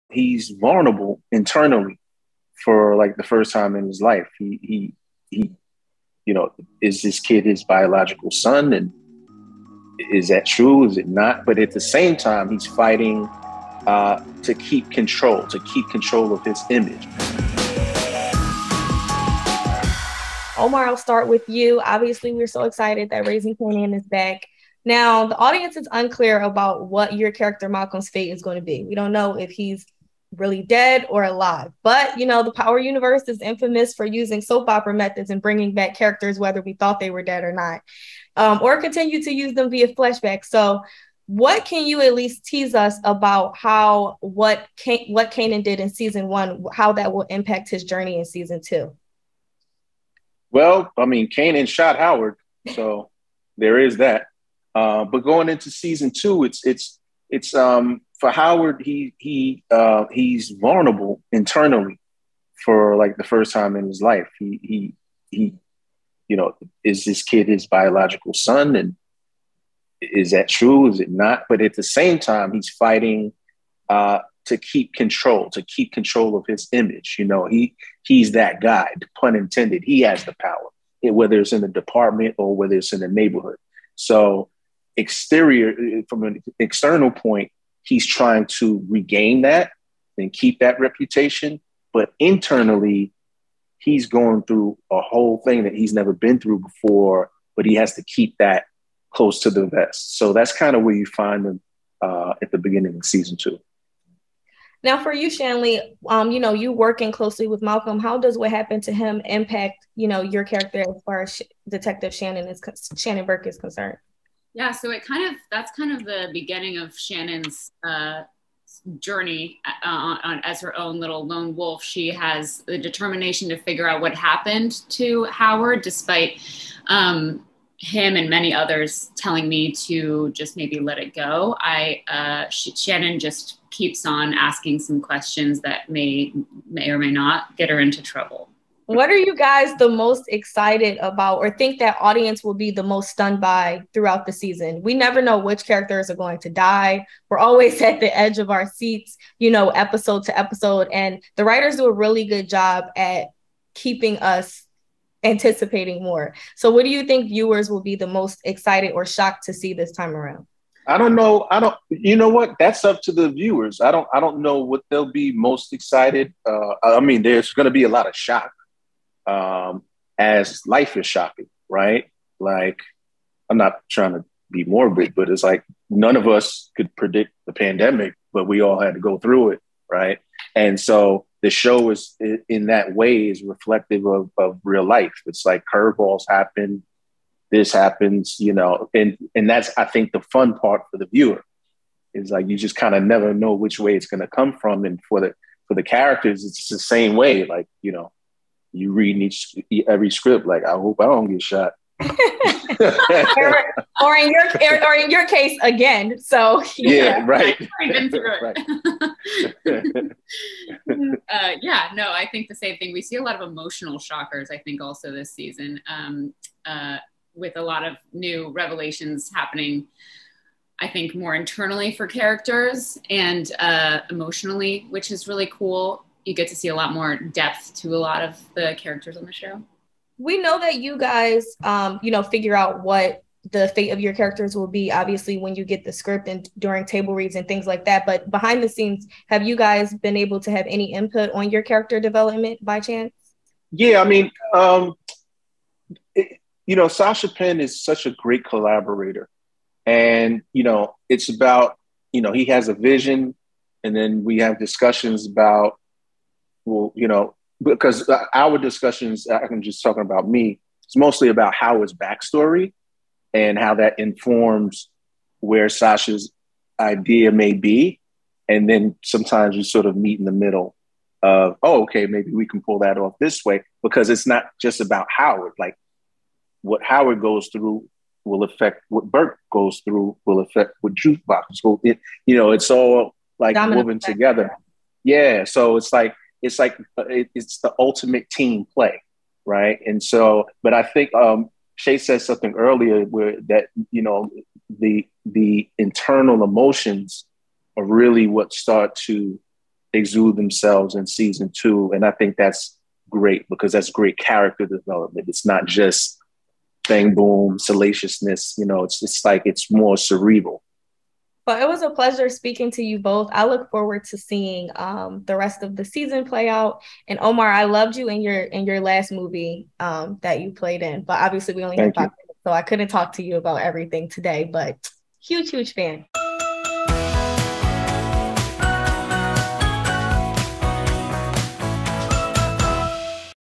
Raising Kanan | Omar Epps and Shanley Caswell on Det. Howard and Burkes Fates in Season 2
Raising Kanan is back for a second season, and has already been green-lit for a third season ahead of its Season 2 premiere on STARZ. The third series in the Power Universe family centers on the original story of the fan-favorite character, Kanan Stark, and follows his coming of age in South Jamaica, Queens. Set in the 1980s and raised by a single mother, who ruthlessly runs the family's emerging criminal empire using her son at the helm, Kanan charts out a path through some of the city's most volatile streets while, at the same time, navigating his way through adolescence, which has its own dangers and pitfalls.

Alongside its stars, MeKai Curtis in the titular role of and Tony® Awards winner Patina Miller as his mother, Raquel "Raq" Thomas, the show also stars Love & Basketball standout Omar Epps as Detective Malcolm Howard and Shanley Craswell as Detective Shannon Burke, Howard's partner on the force. Season 1 ended with Det. Howard coming to grips with the news that Kanan could be his son, and Kanan shoots him under the direction of his own mother.  In an exclusive with PopCulture.com host Brenda Alexander, Omar Epps and Shanley Caswell tease what audiences can expect from this season.

For more on RaisingKanan, OmarEpps and Starz, head to PopCulture.com for the latest in entertainment news, streaming news, TV news and updates.